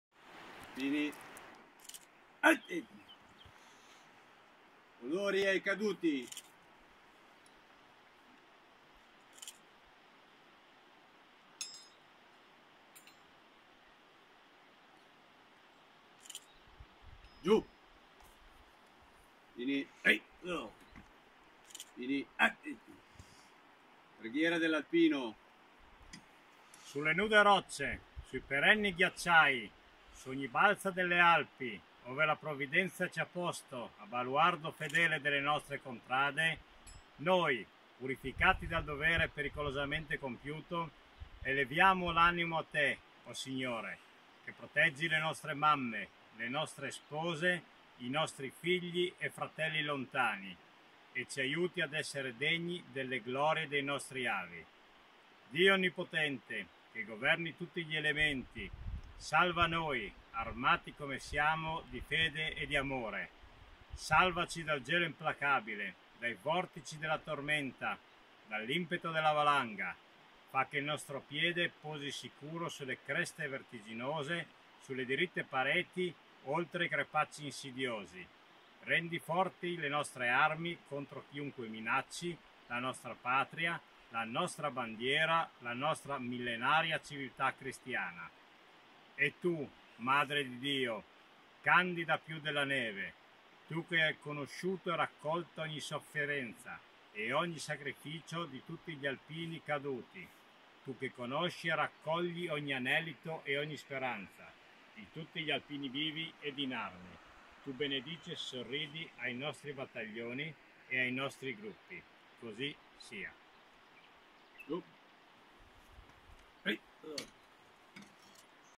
istoritravel Cerimonia in onore ai Caduti al Ponte di Perati
Breve cerimonia di commemorazione in memoria di tutti i caduti italiani e greci della Campagna di Grecia sul ponte di Perati.
Un plauso e i nostri complimenti alle sezioni Alpini di San Giorgio di Nogaro, Palmanova, Trieste e Cividale del Friuli